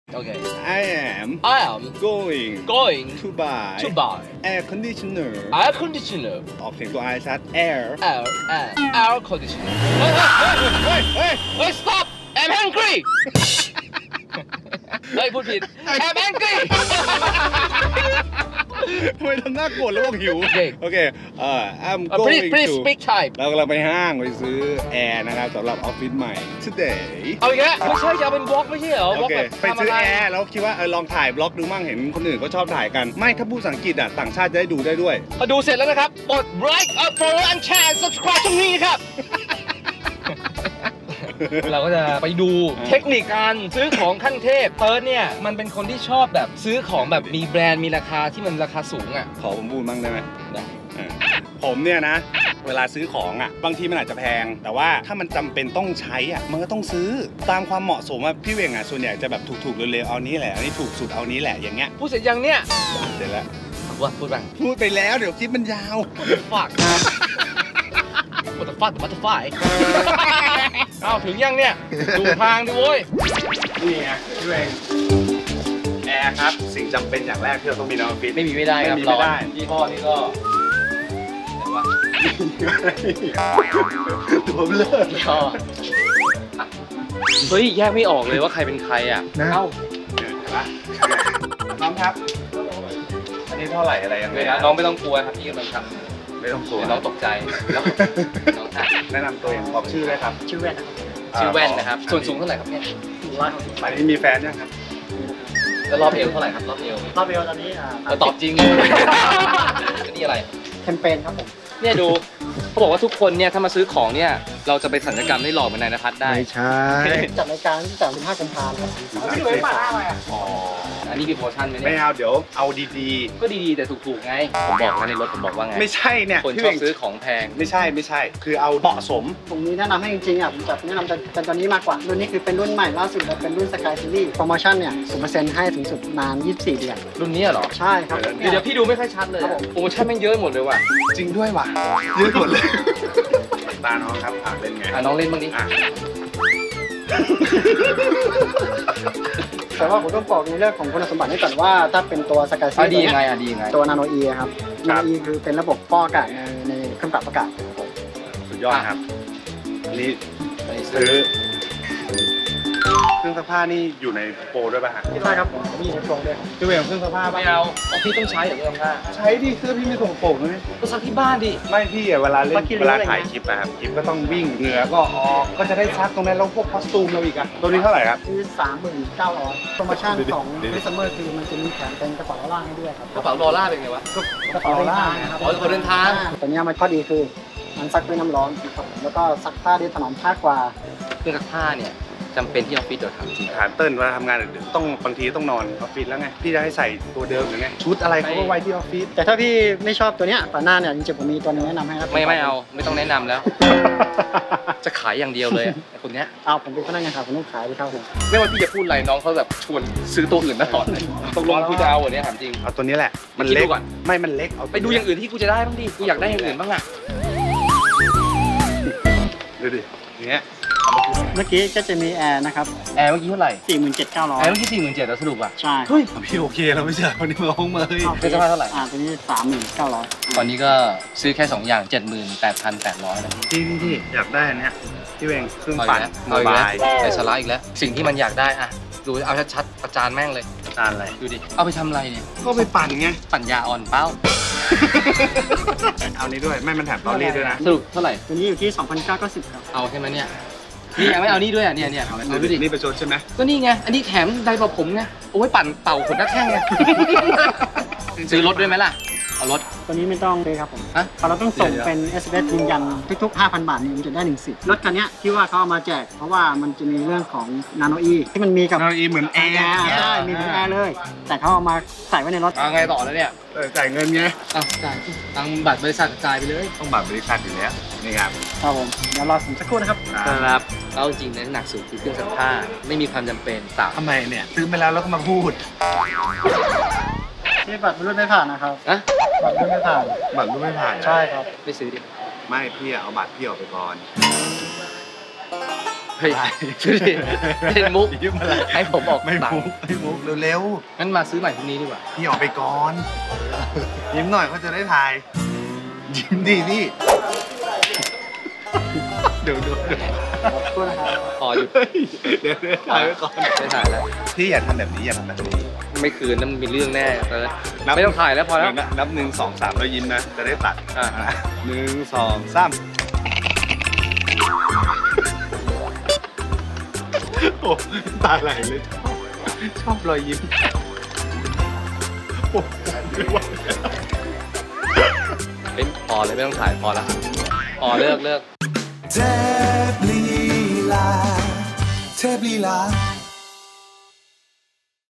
ซื้อของเข้าออฟฟิศใหม่ VLOG EP.1 | เทพลีลา
ไปซื้อของเข้าออฟฟิศใหม่ พี่เหว่งโคตรเรื่องมากเลย ใช้เงินไม่ค่อยเป็น ของที่ต้องใช้ก็ไม่อยากจะซื้อ เบื่อ.. ลองไปดูพฤติกรรมของพี่เหว่งในคลิปดูครับทุกคน

กดSubscribeเพื่อที่จะได้ไม่พลาดคลิปใหม่ๆจากเทพลีลา